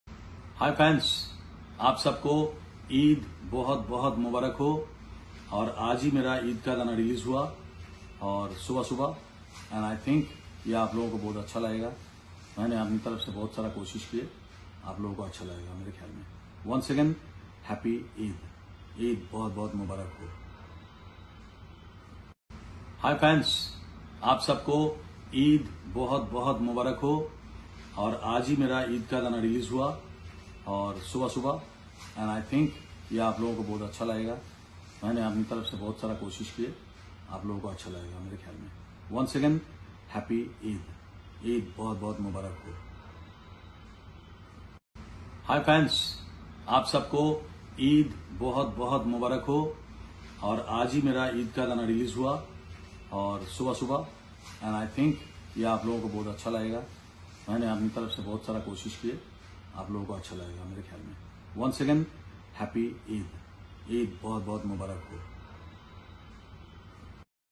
Hyderabad....Singer Kumar Sanu Ki Janib Se Ap Sab Ko Advance Mein Eid Mubarak
Hyderabad.

Singer Kumar Sanu Ki Janib Se Ap Sab Ko Advance Mein Eid Mubarak